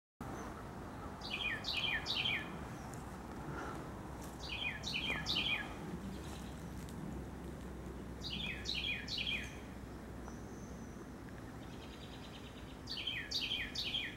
Carolina Wren in Upstate NY
Just a quick video to share with friends, the sound of a Carolina Wren outside my sister's Upstate NY home